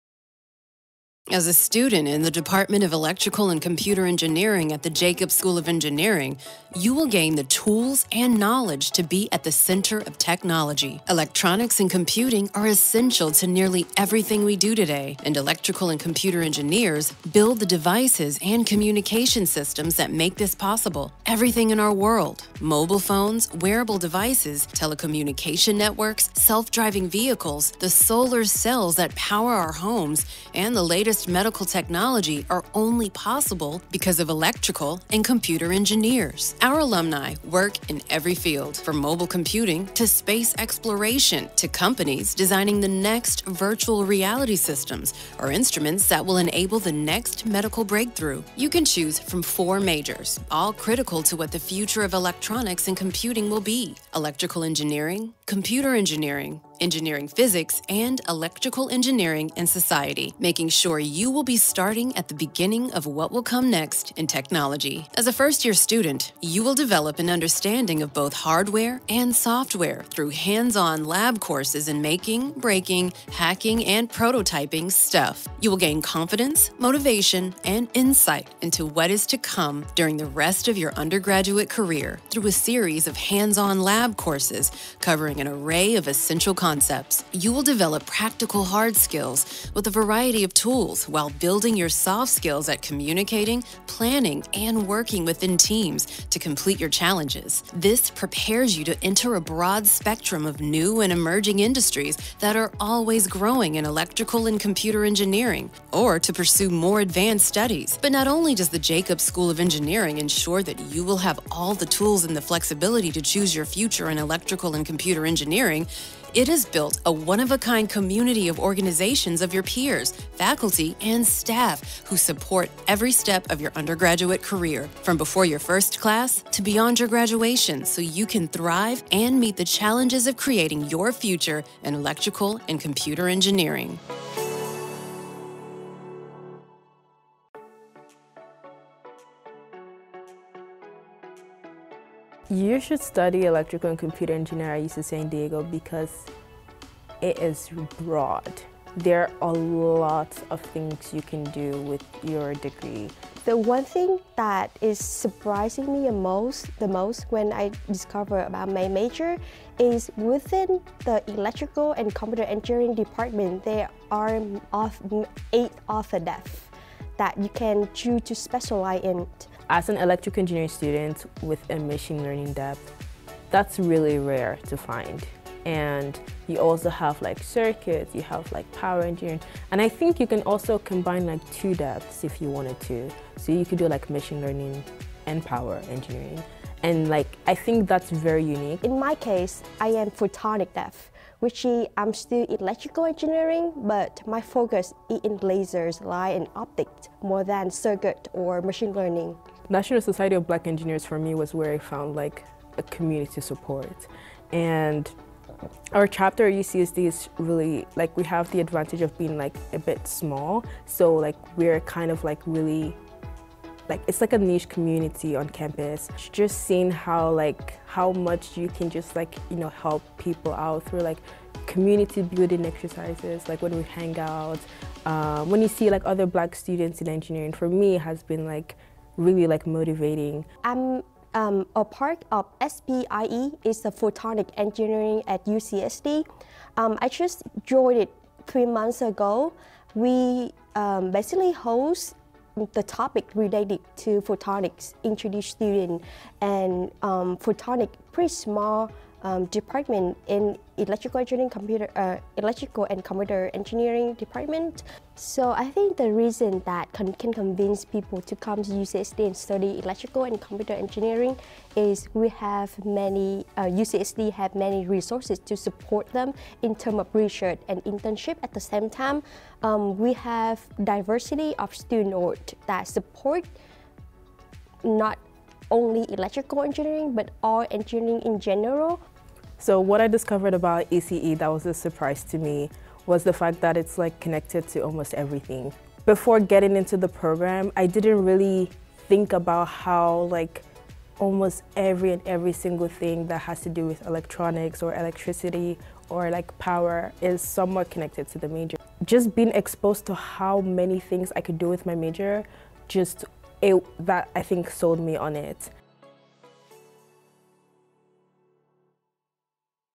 Electrical and computer engineering at UC San Diego
What exactly is electrical and computer engineering? This video explains what undergraduate students who study electrical and computer engineering at UC San Diego will learn; what careers they'll be prepared for; and how electrical engineers use these skills to make an impact in a variety of fields.

For more information on electrical and computer engineering at UC San Diego,